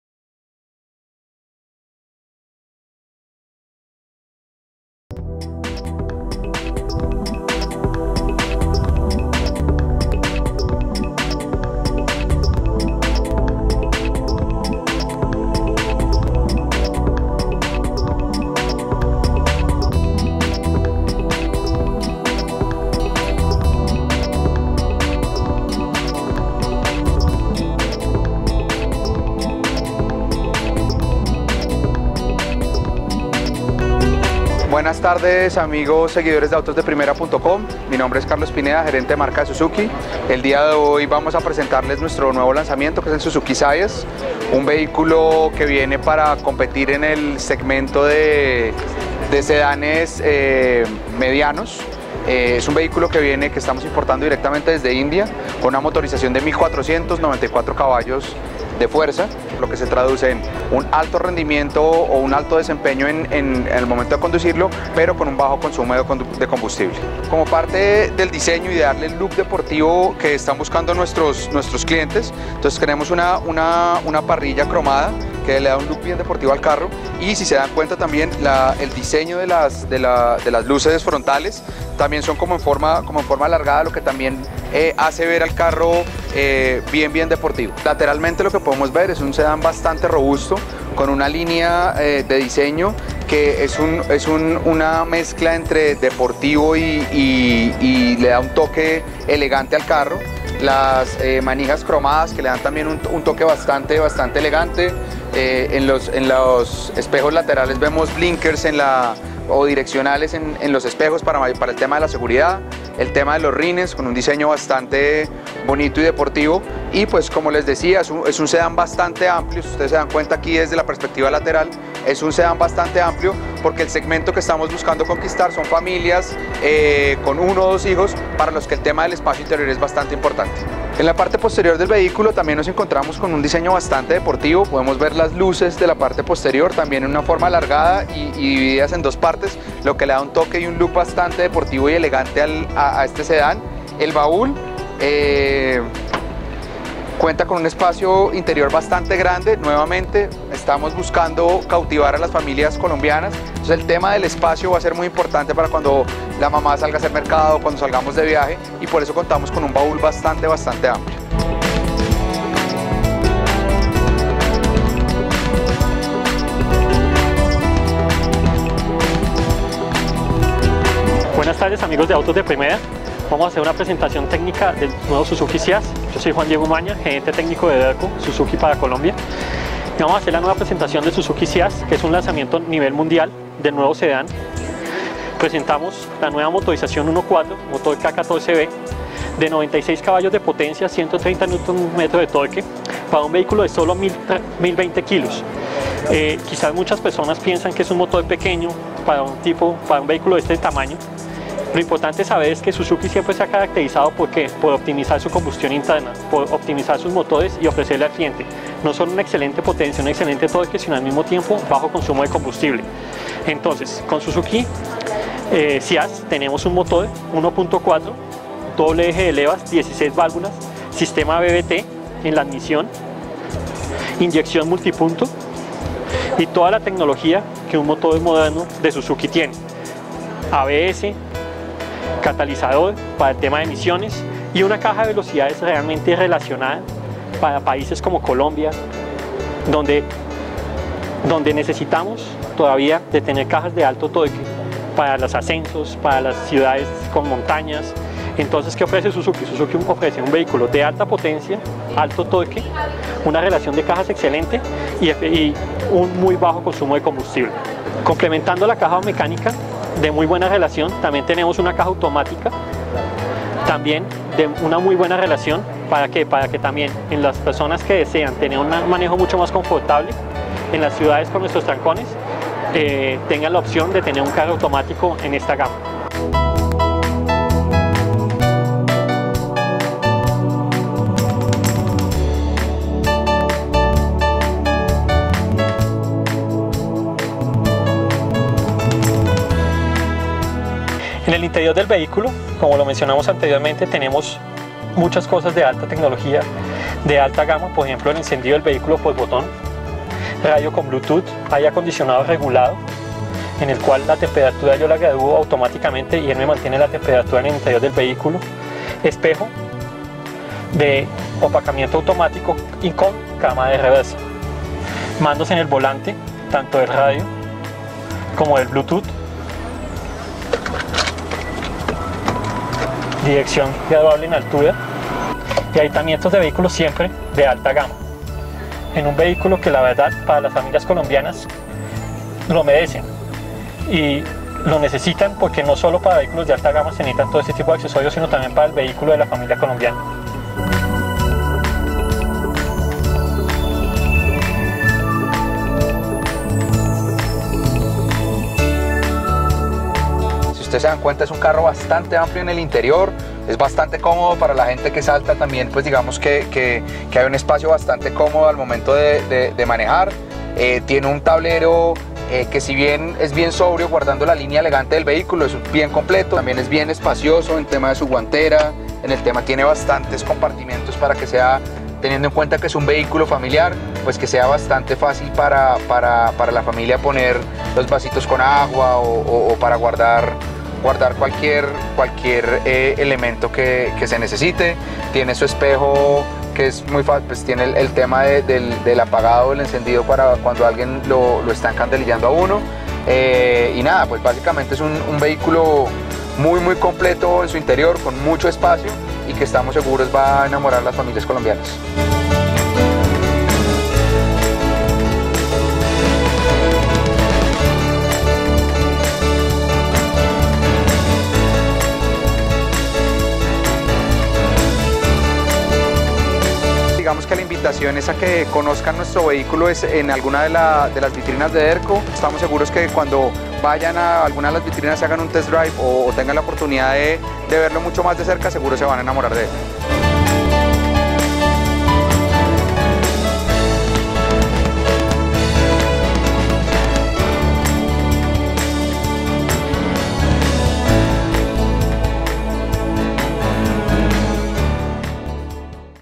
Nuevo Suzuki Ciaz en Colombia - Lanzamiento
El Suzuki Ciaz llega a Colombia y entra a competir directamente en el segmento de los sedanes medianos con un diseño atractivo, perfil moderno y deportivo, además de un precio muy seductor.